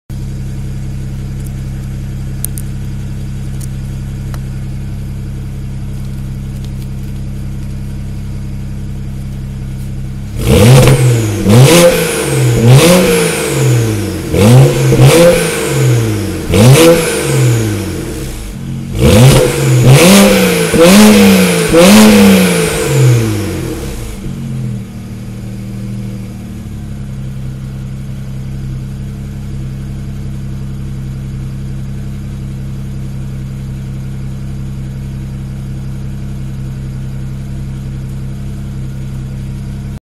BMW 335i Downpipe Revving
BMW 335i Coupe Stock exhaust and AR Designs Catless Downpipes. Cold Revs, to 4000 RPM Max. JB4 Can-Flap open option activated.